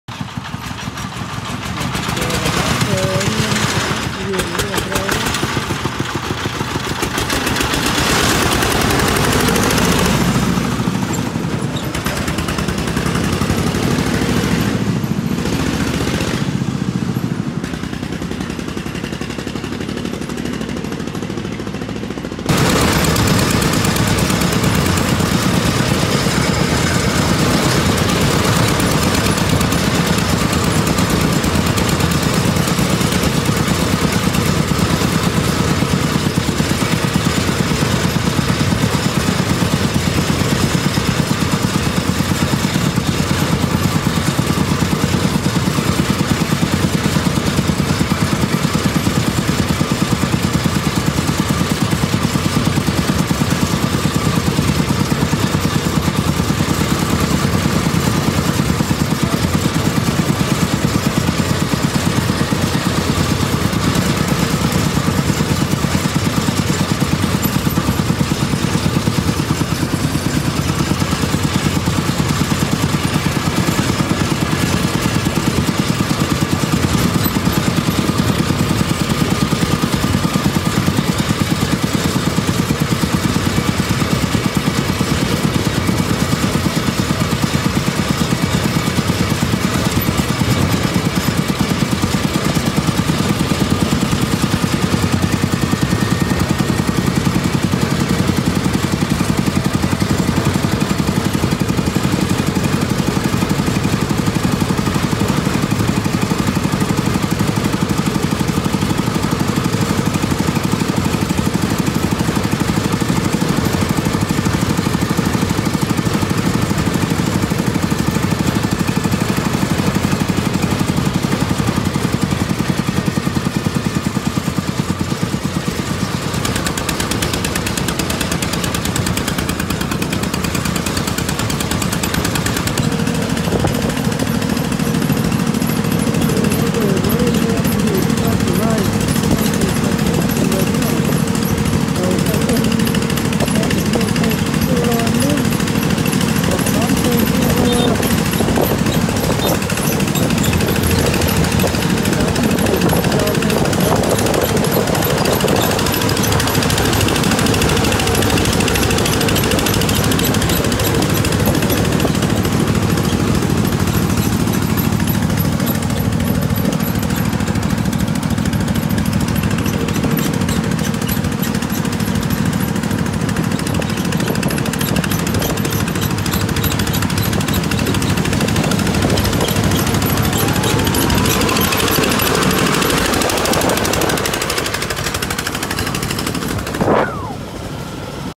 Đâu Năm Khai Xuân Đã Bán Thành Công Chiếc Công Nông 2 Cầu Giá 75Tr
Mung 6 Tết Đã Bán Thành Công Chiếc Công Nông 2 Cầu.. Chúc AE Về Chạy Tốt Dùng Bền, Đâu Năm Cảm Ơn AE Đã Nhiệt Tình Ửng Hộ. AE Cần Mua Xe Gì Cứ Alo Em ❤️🥰